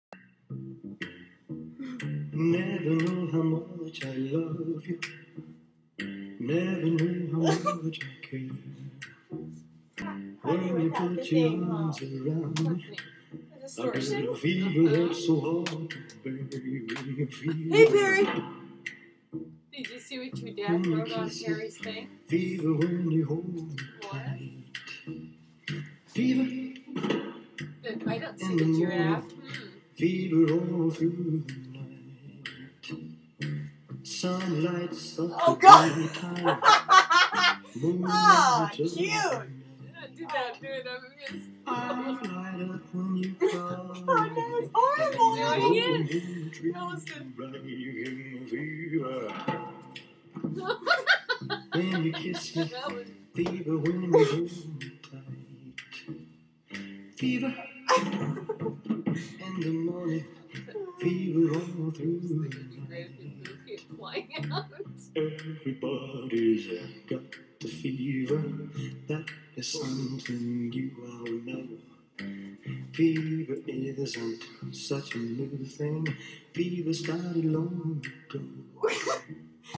Strange Fever Dance
This is me happy and having fun with my mim. We have the best time together, and I imagine me messing around like this has a lot to do with it. Don't mind the part where my skirt goes up. **hahaha**